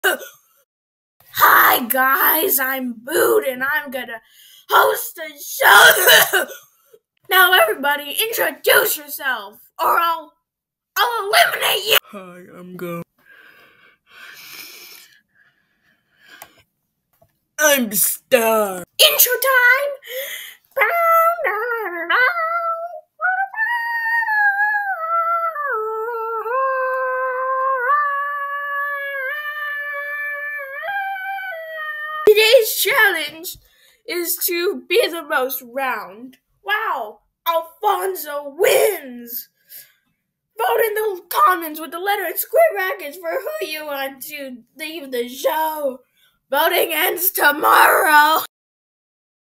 ARBOS|| EP. 1
ARBOS is an acronym for “A Really Bad Object Show” it was made horribly on purpose. If gum sounds familiar that’s because gum is voiced by @Corgithunder3!